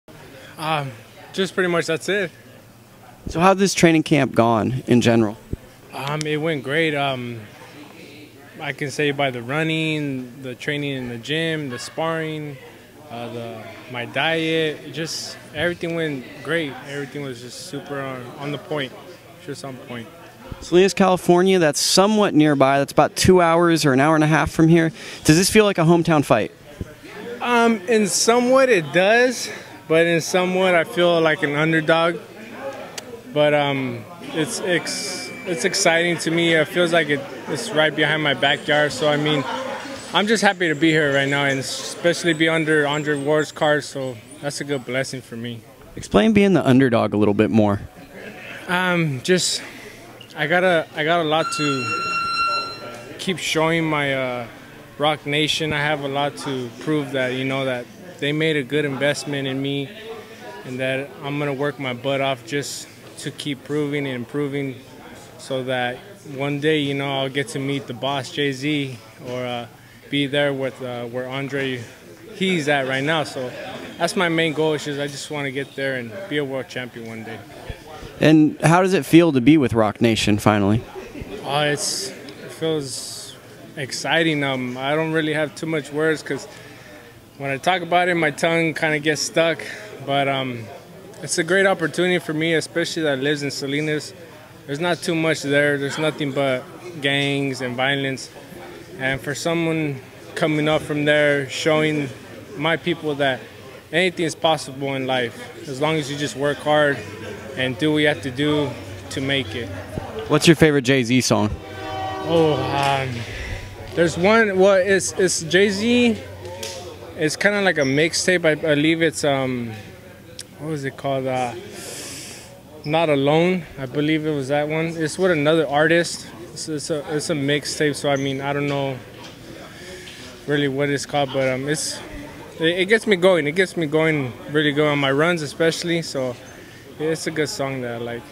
Rudy Puga media day highlights
Rudy Puga Jr. of Salinas, CA media day highlights for Roc Nation card on Oakland, CA on March 26th